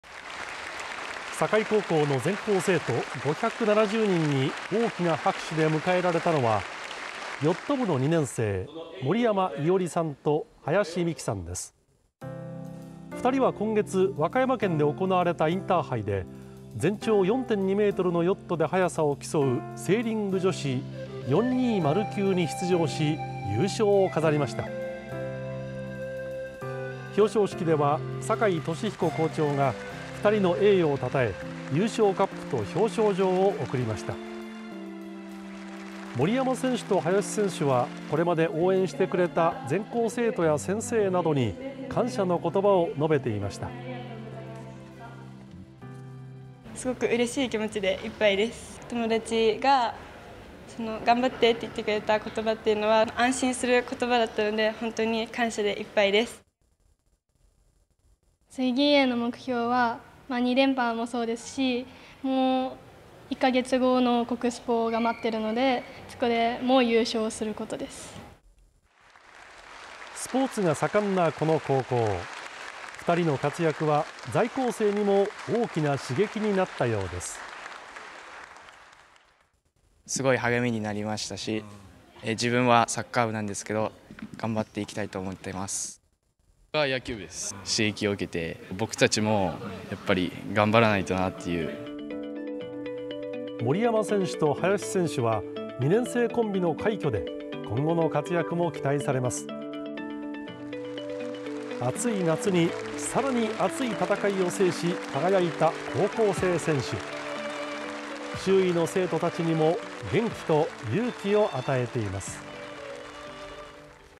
【全国優勝】インターハイセーリング女子優勝の境高校2年生ペアが母校で表彰 在校生にも大きな刺激 鳥取県境港市
全国優勝の快挙です。全国高校総体、インターハイのセーリング女子で優勝した境高校の2年生のペアが8月26日に母校で全校生徒を前に表彰を受けました。

境高校の全校生徒570人に大きな拍手で迎えられたのは、ヨット部の2年生、森山伊織さんと林深姫さんです。2人は8月に和歌山県で行われたインターハイで全長4.2mのヨットで速さを競う「セーリング女子420級」に出場し、優勝を飾りました。表彰式では酒井敏彦校長が二人の栄誉をたたえ優勝カップと表彰状を贈りました。

森山選手と林選手は、これまで応援してくれた全校生徒や先生などに感謝の言葉を述べていました。

森山伊織 選手
「（全校表彰され）すごくうれしい気持ちでいっぱいです。友達が（レース前に）”頑張って！”って言ってくれた言葉は安心する言葉だったので本当に感謝でいっぱいです」

林深姫 選手
「次への目標は、二連覇もそうですし1か月後”国スポ”が待っているのでそこでも優勝することです」

スポーツが盛んなこの高校。2人の活躍は、在校生にも大きな刺激になったようです。

在校生
「すごく励みになりました。自分はサッカー部だけど頑張っていきたい」

「野球部です。刺激も受けて僕たちも頑張らないと」

森山選手と林選手は、2年生コンビでの快挙で今後の活躍も期待されます。
暑い夏にさらに熱い戦いを制し輝いた高校生選手。周囲の生徒たちにも元気と勇気を与えています。